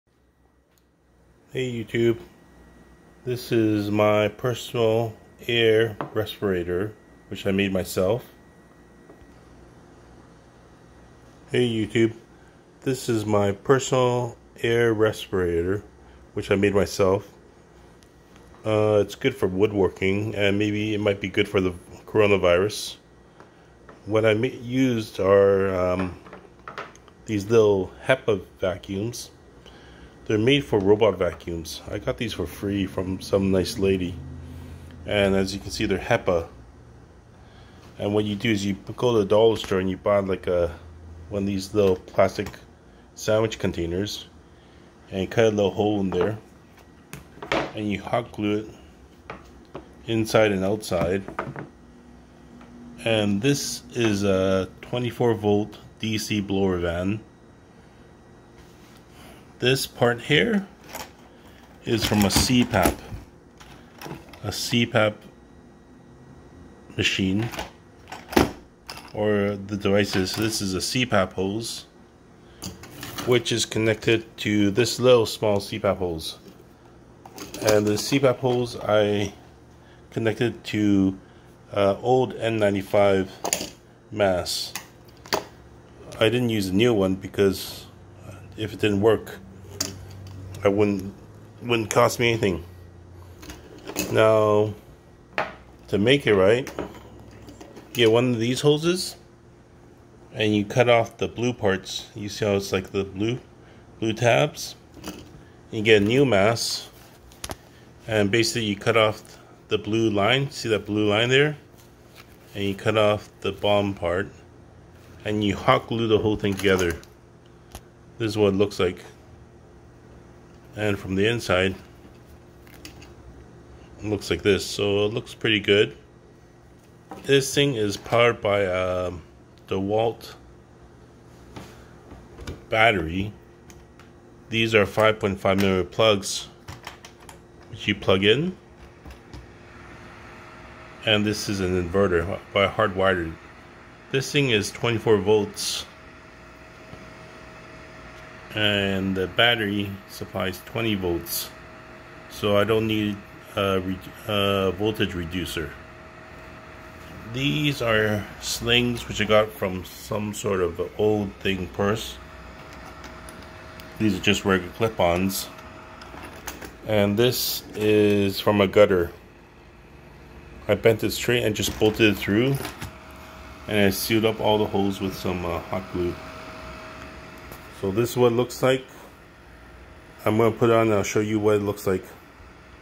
DIY PAPR 24 volt dc blower fan. no ESC. no arduino, cheap and fast .coronavirus covid-19 face mas
This  PAPR uses  a robot vacuum HEPA filter.  a 24 volt dc blower and cpap hoses and a N95 MASK.  it should be good for wood working, or if your a bus driver, or uber driver.   The HEPA filter is not rated for use as a respirator, but they are cheap and sometimes you can't find P100 filter.  some joker was selling 20 pairs of P100 filter for 1000 bucks. thats like 50 bucks a filter and crazy expensive.   I have a problem editing and putting 2 videos together.